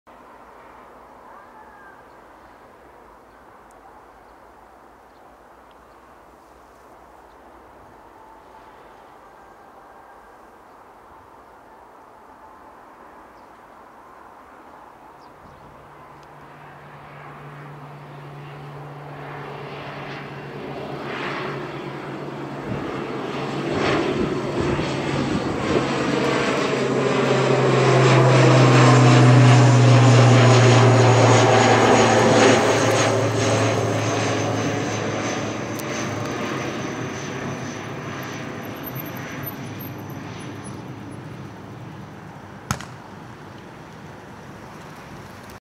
하이에어(HL5244) ATR 72-500 울산공항 이륙
하이에어 ATR 72-500 울산공항 이륙 모습입니다.

Hi-Air HL5244 departing RKPU(USN) at 25 Jun 2023